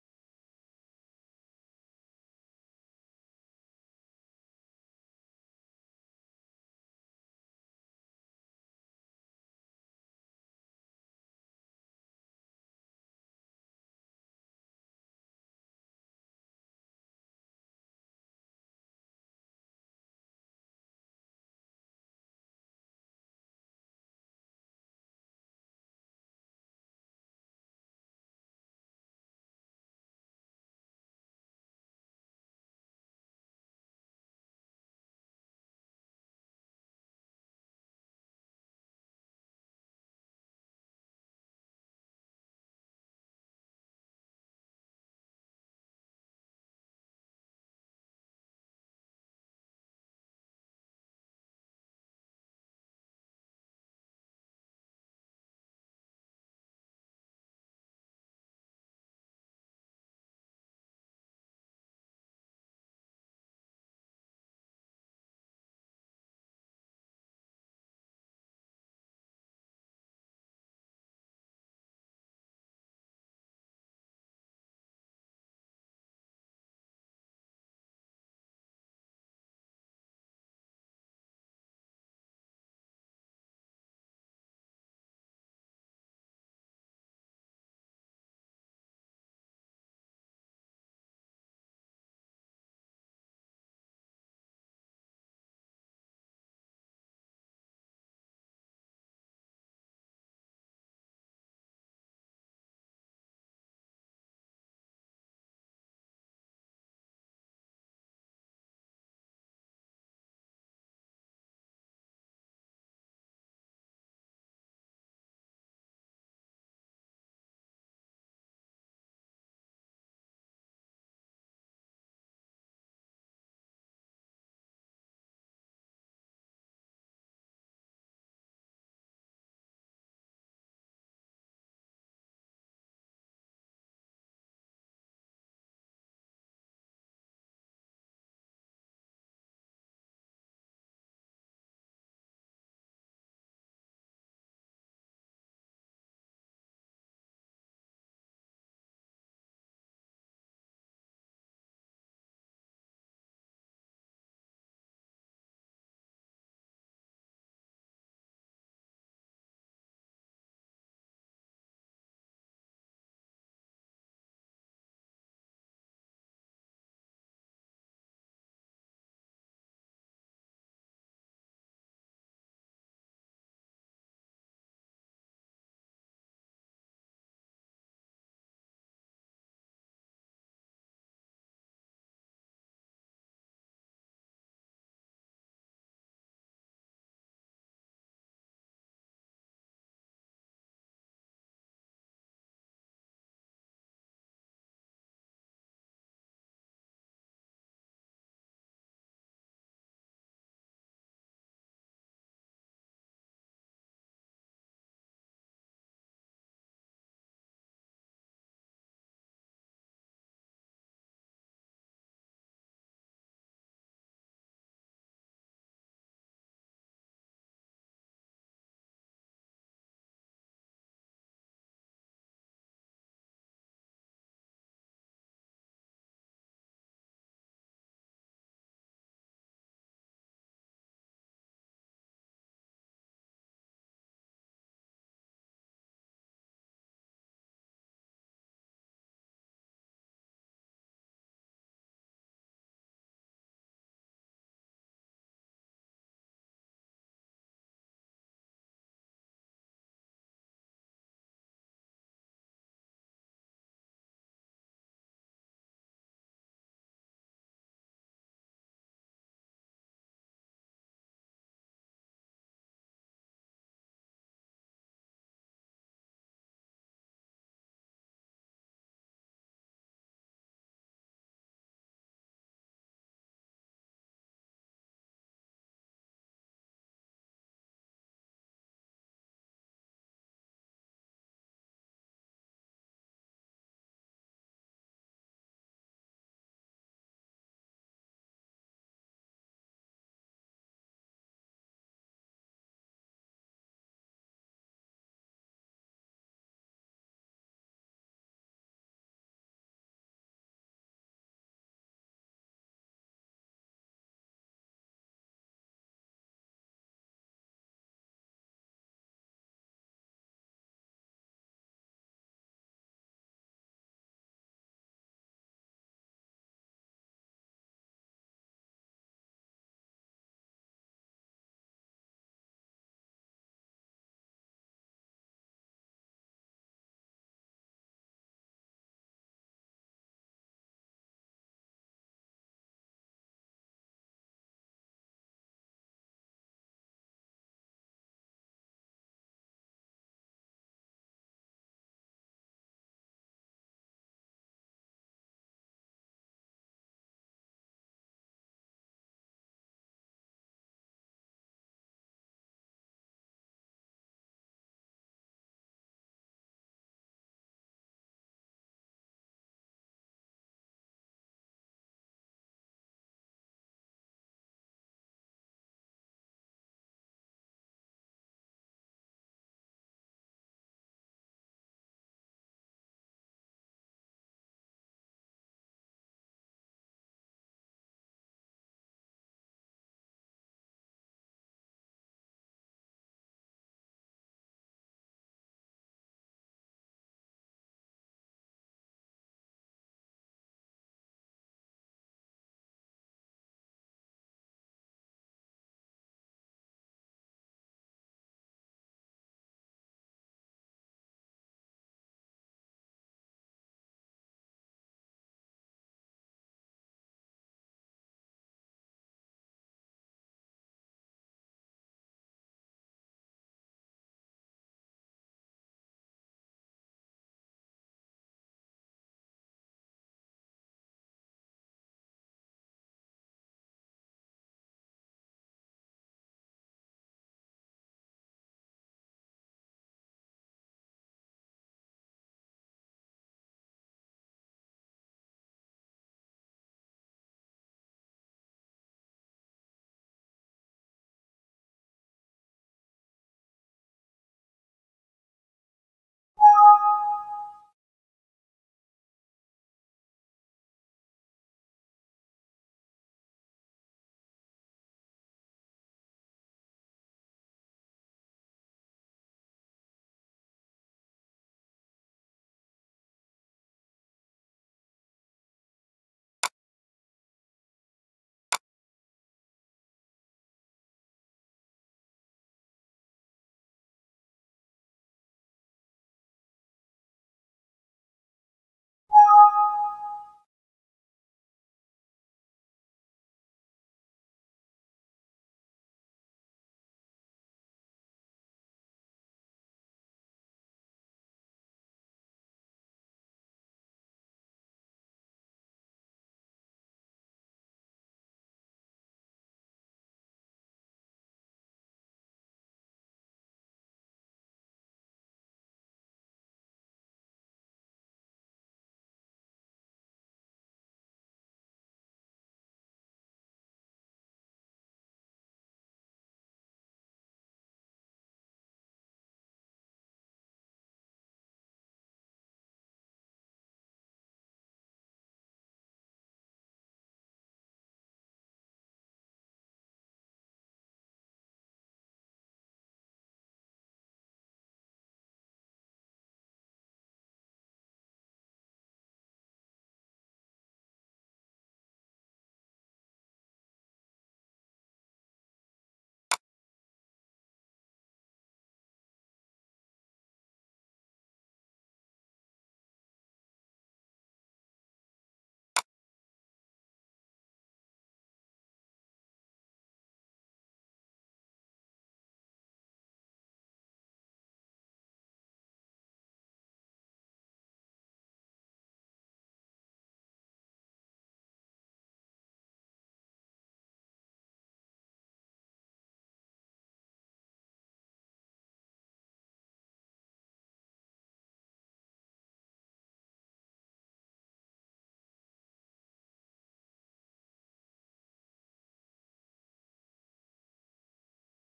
World of Goo Chapter Tutorial, Part 1: Making the Map
This is a tutorial for how to create your very own chapter in World of Goo! In Part 1, we look at how to design the map.